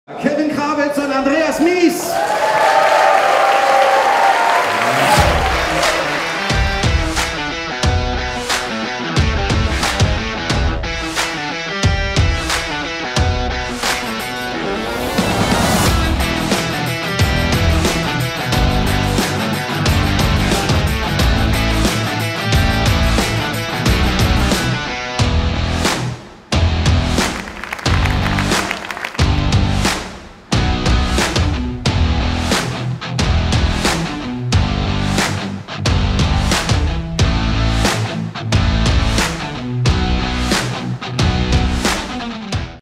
Auslosung der Noventi Open | Noventi Open 2019 | myTennis
Verfolgt die Auslosung der Noventi Open mit den beiden Losfeen
Kevin Krawietz und Andreas Mies. Gegen wen Roger Federer und Alexander Zverev bei den Noventi Open spielen, seht ihr genau hier.

Jetzt für iPhone und Android herunterladen (nur für Deutschland):
iPhone: